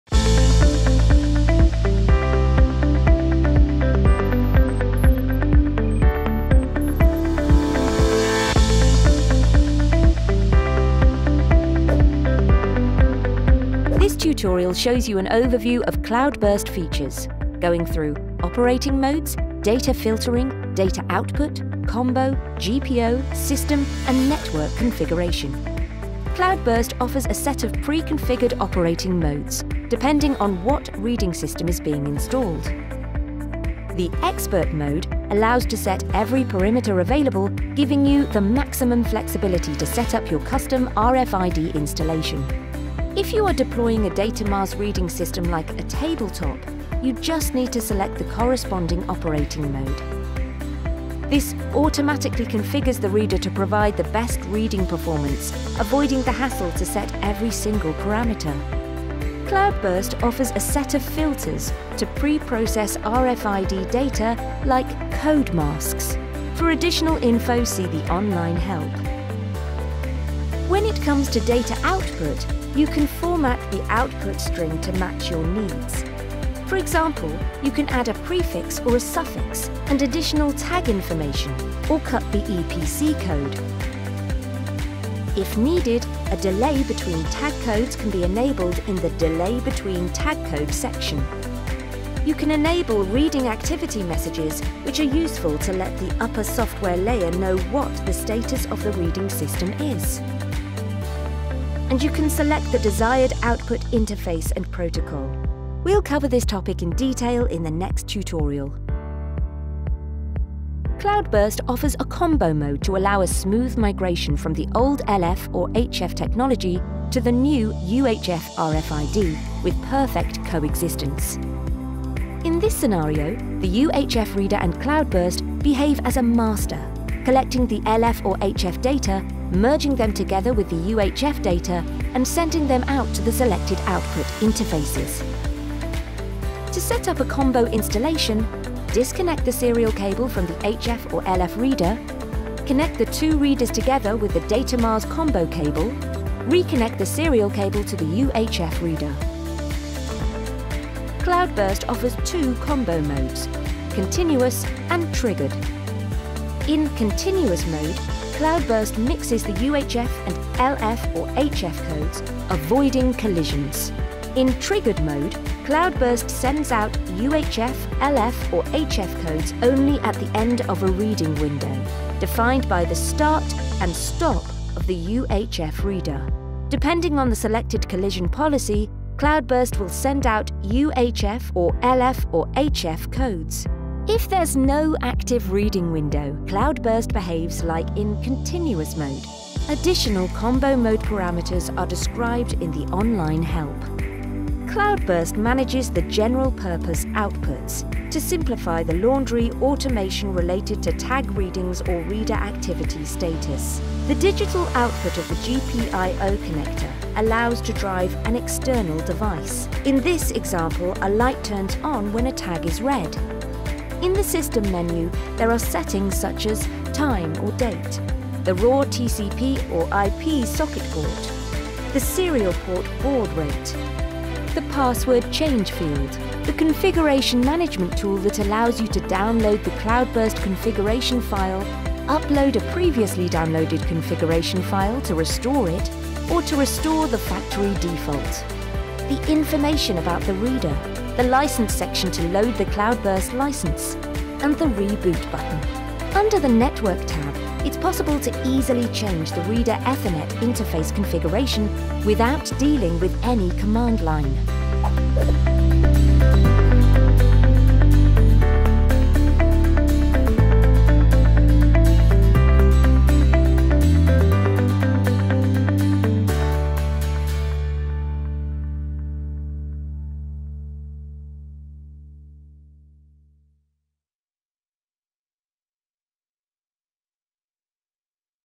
Cloudburst tutorial nr. 3 _ Features
This tutorial shows you an overview of Cloudburst features, going through: operating modes, data filtering, data output, combo, GPO, system and network configuration.